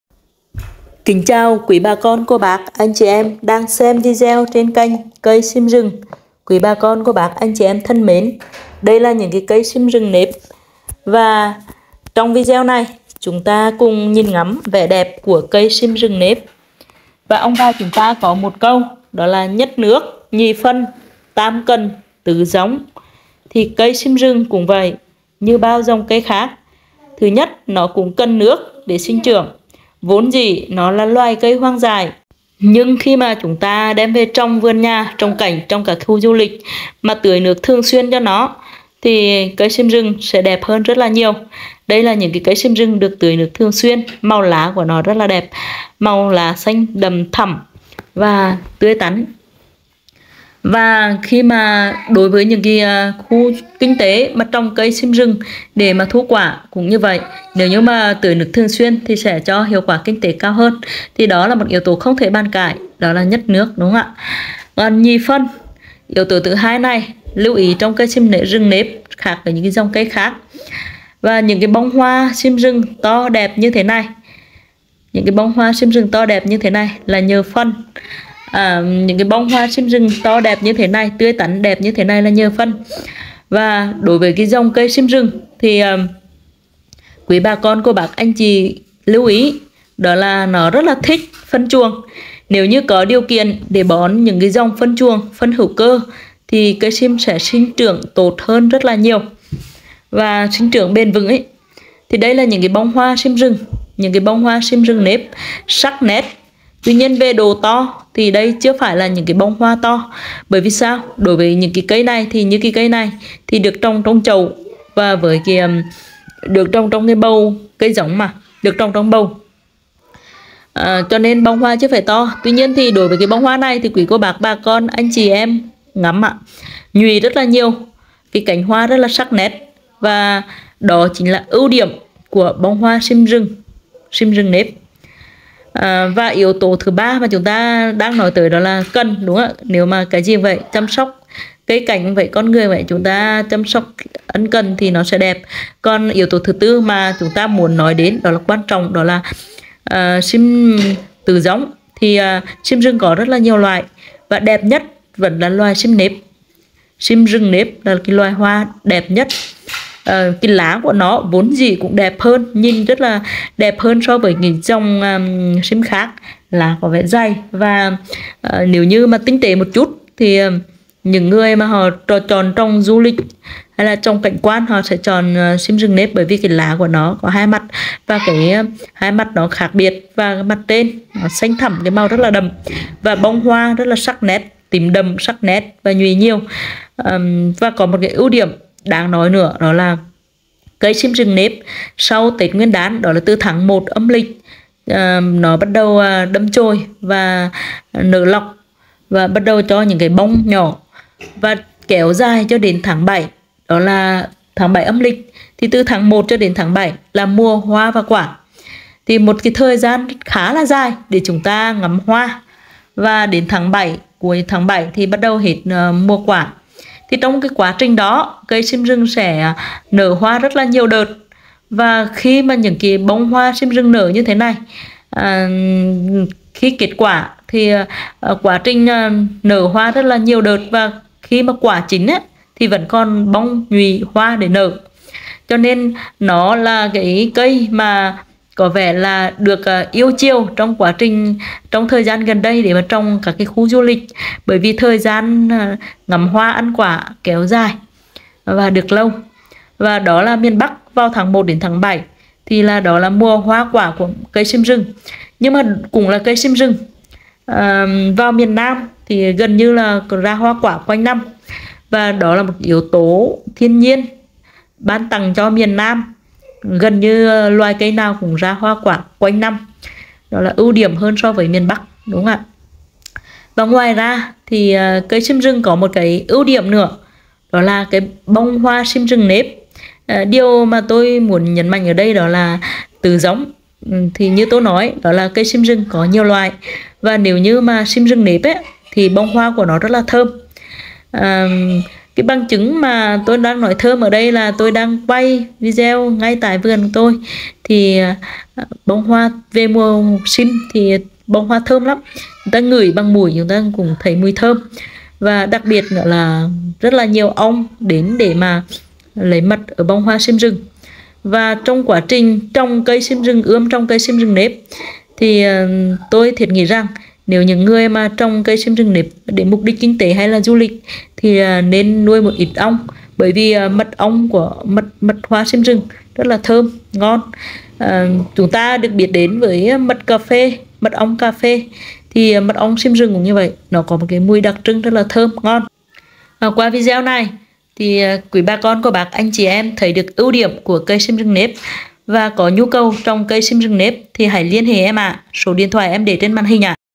Những điều thú vị khi trồng cây sim rừng từ nhà vườn chuyên cung cấp cây giống sim rừng
Cây sim rừng cũng như bao cây trồng khác cần được tưới nước và chăm sóc cây trồng sẽ đẹp hơn, cho hiệu quả kinh tế cao hơn. Sim rừng có mấy loại, trong đó cây sim nếp là loại cho nhiều hoa quả, đẹp nhất, hiệu quả kinh tế cao nhất.